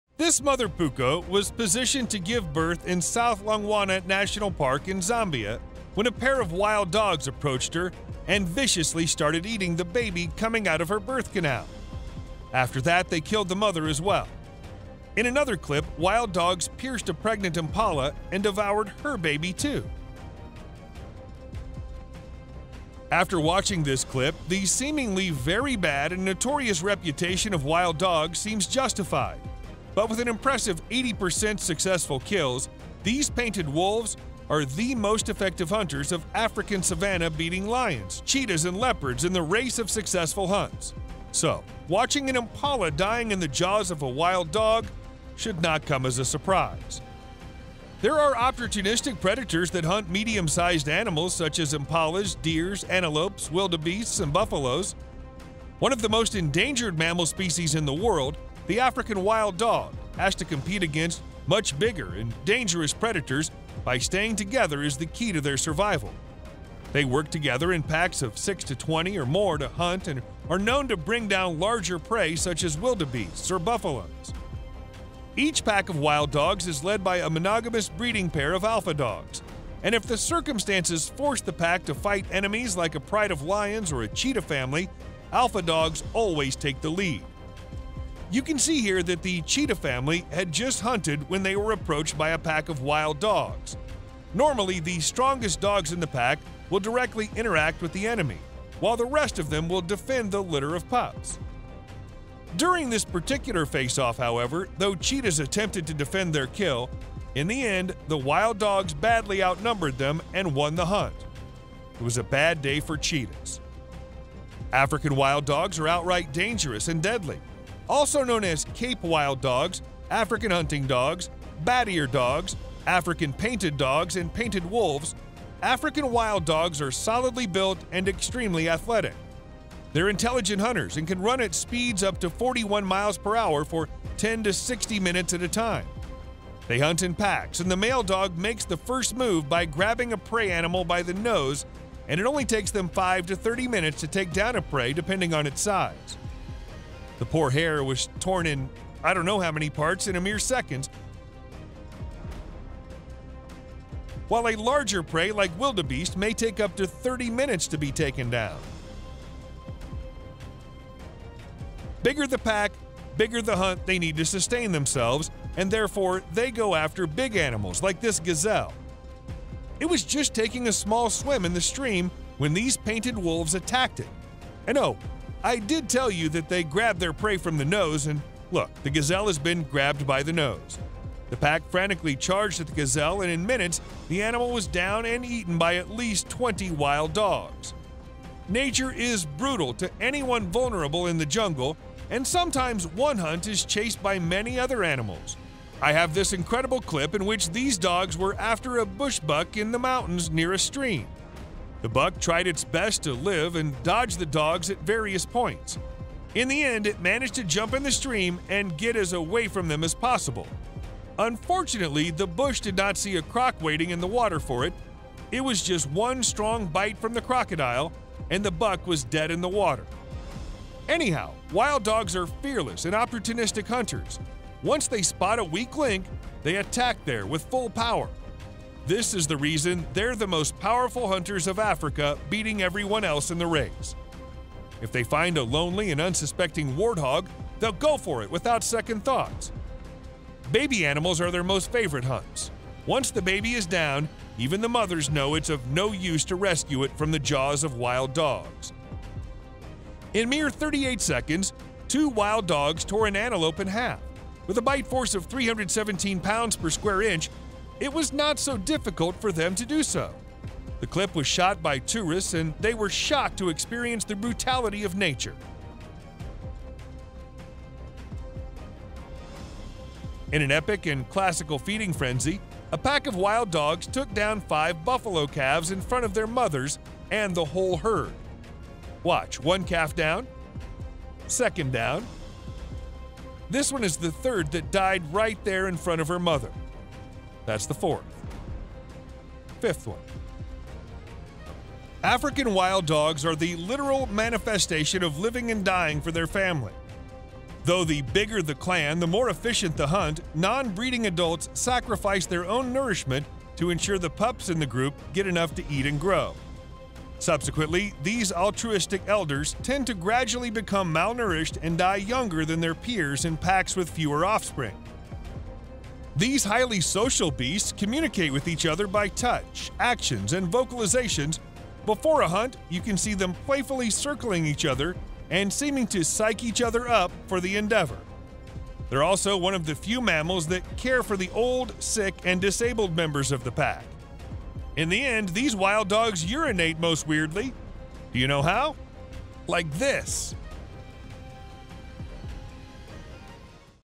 15 HORRIFIC Ways Wild Dogs Kill Their Prey
We hope you enjoy our documentary on amazing animals that nature provides us. A lot of work has gone into the making of this video.

Impalas dying in a wild dogs jaw isn't the only animal that gets ripped to pieces by these predators. Many times nature shows us that we live in a dog eat dog world and that really hits home when we see new videos of how nature is at work even when we sleep.